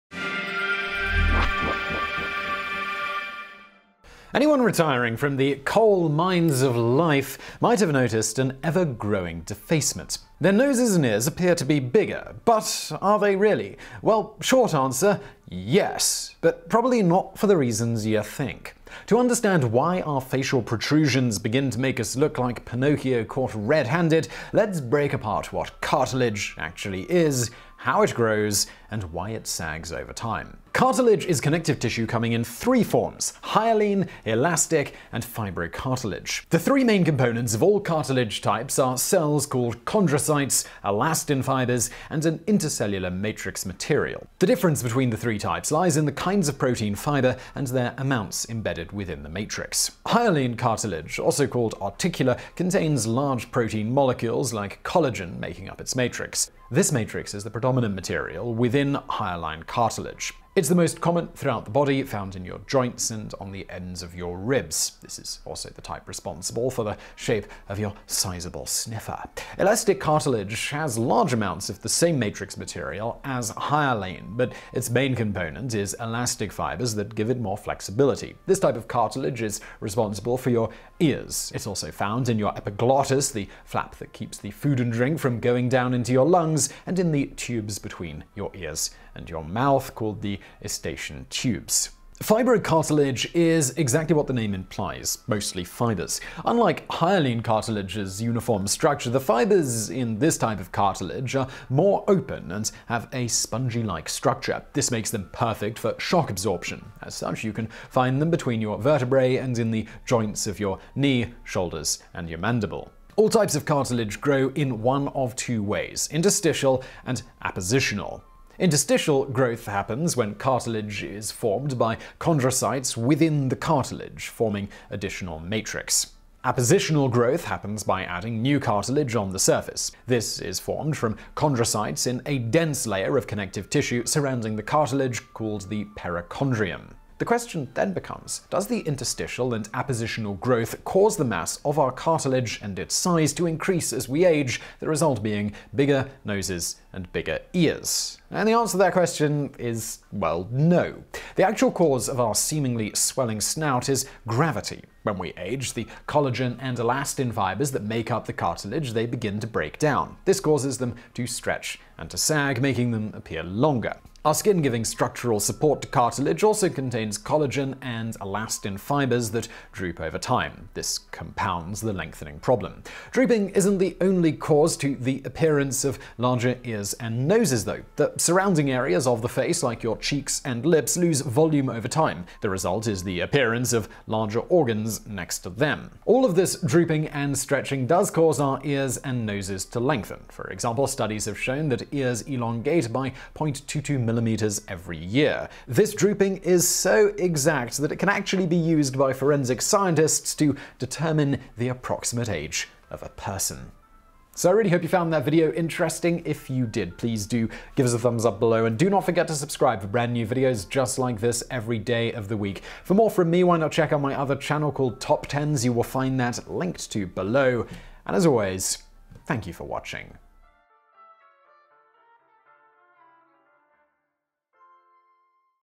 Do Your Ears And Nose Really Continue To Grow As You Age
In this video:

Anyone retiring from the coal-mines of life, might have noticed an ever growing defacement. Their noses and ears appear to be bigger. In fact, they are. There is a common misconception this growth is due to cartilage continuing to grow as you age. In reality, this isn’t true. The real reason our noses and ears keep growing is the result of that red-headed-stepchild of fundamental forces, gravity.